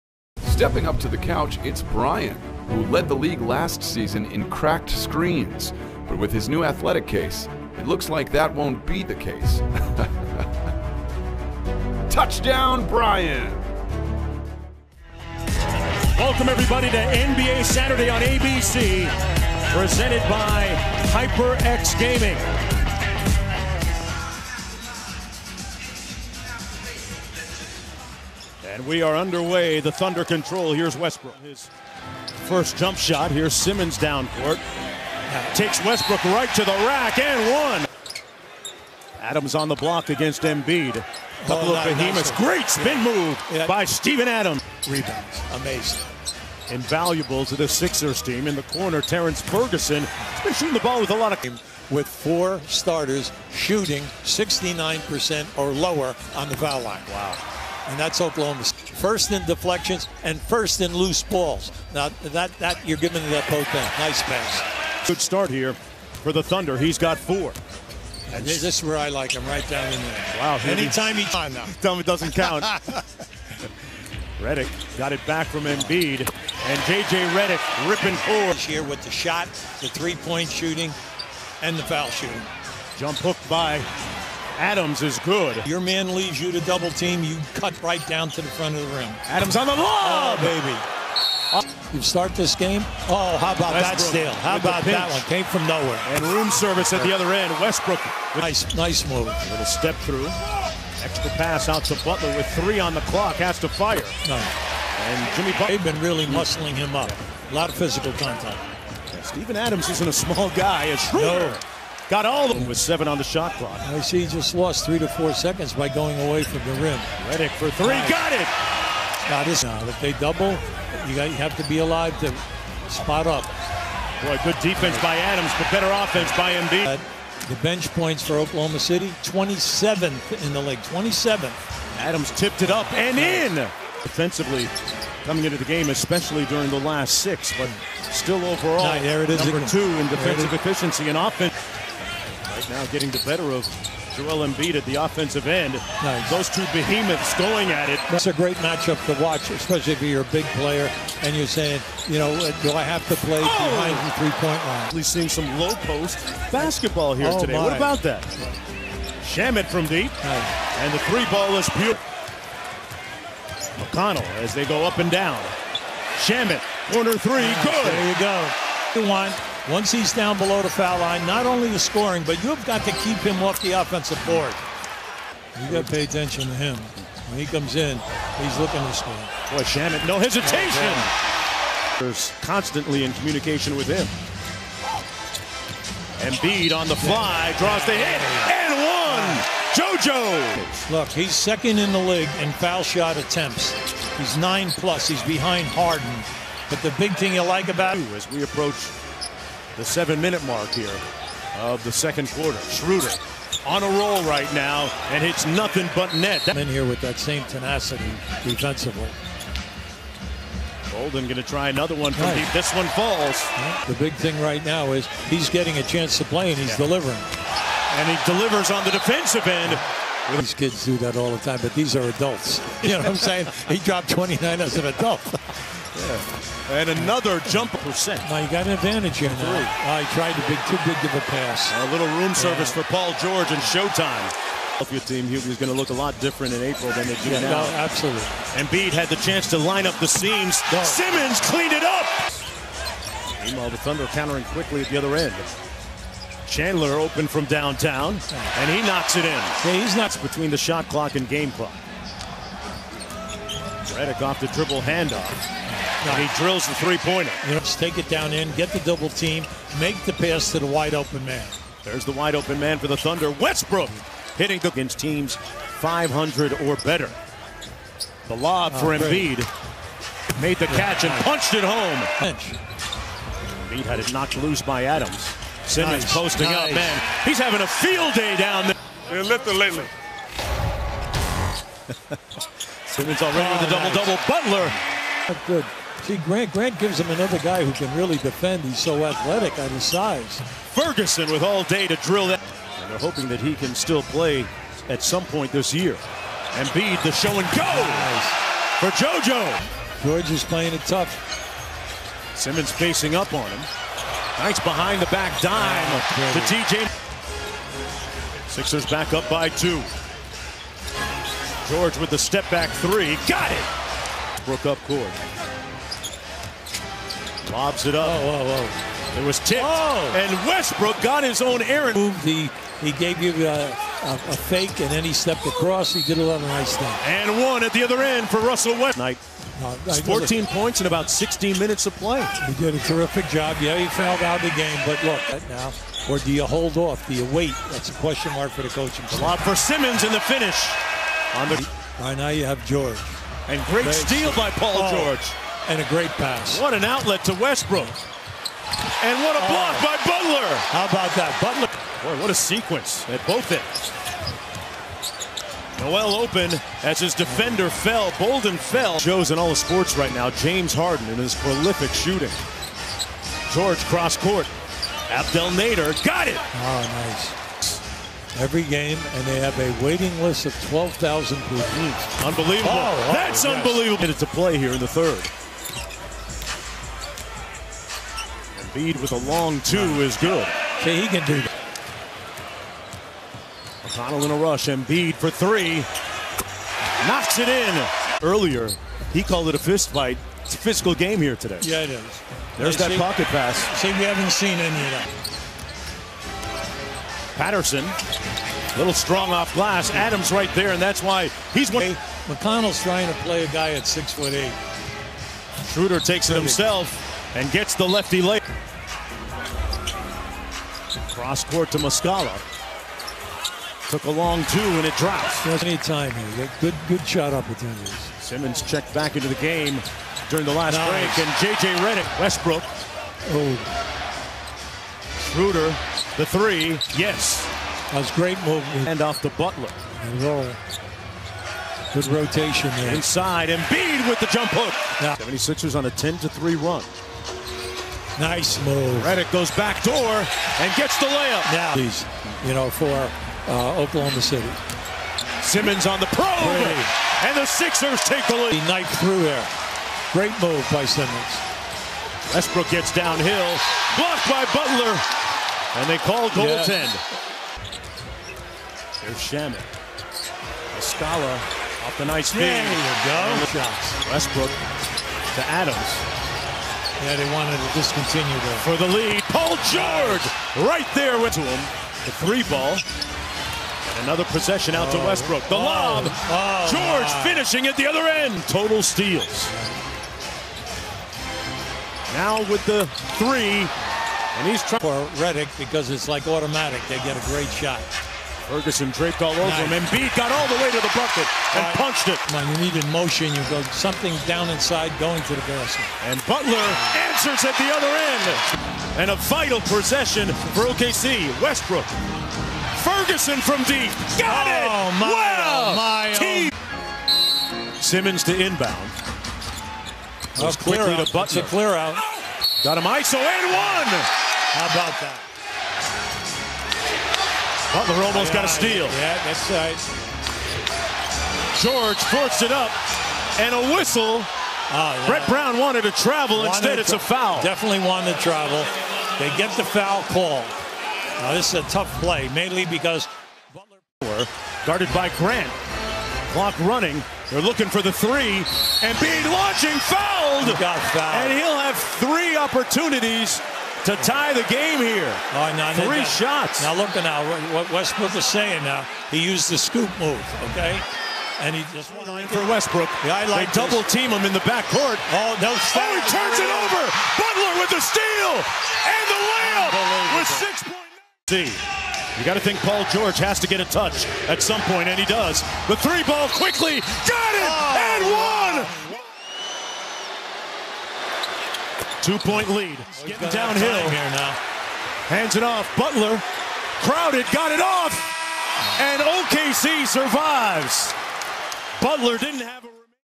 OKC Thunder vs Philadelphia Sixers - Full Game Highlights | January 19, 2019 | 2018-19 NBA Season
Philadelphia Sixers 76ers vs Oklahoma City Thunder - Full Game Highlights | January 18, 2019 | 2018-19 NBA Season

for official scores & highlights.